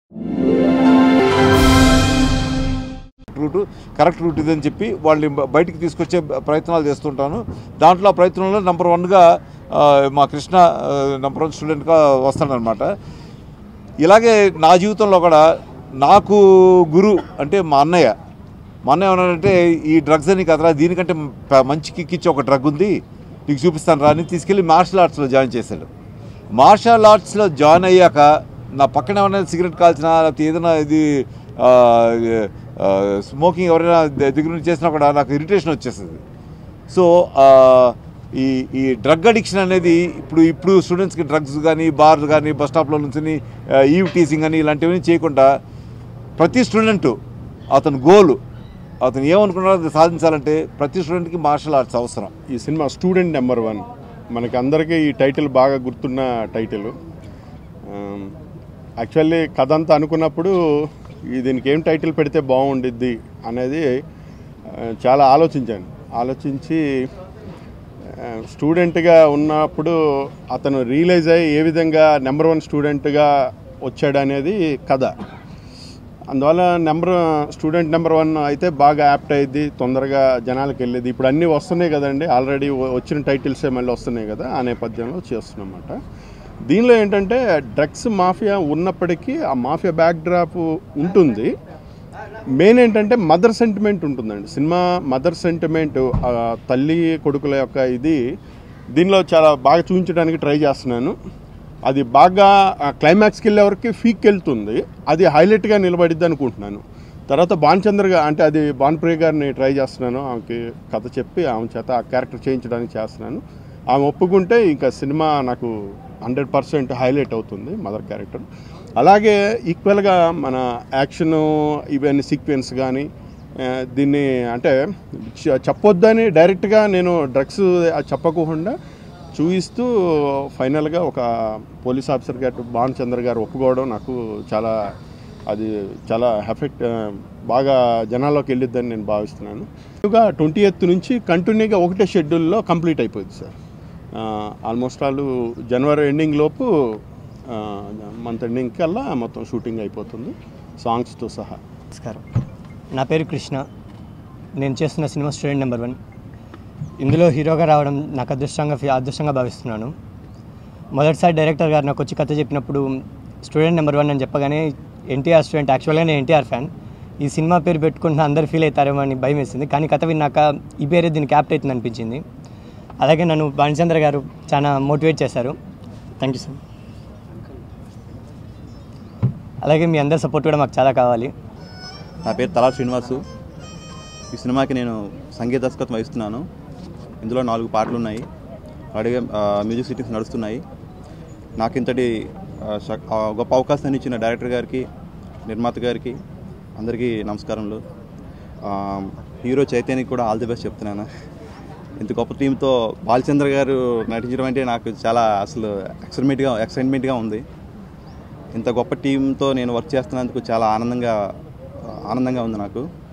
Rakshaka Bhatudu Movie Opening Press Meet || Richapanai, Bahubali Prabhakar || E3 Talkies
Watch Rakshaka Bhatudu Movie Opening Video. directed by Vamsi Krishna Akella. produced by A. Gururaj. starring: Richa Panai & Bahaubali Prabhakar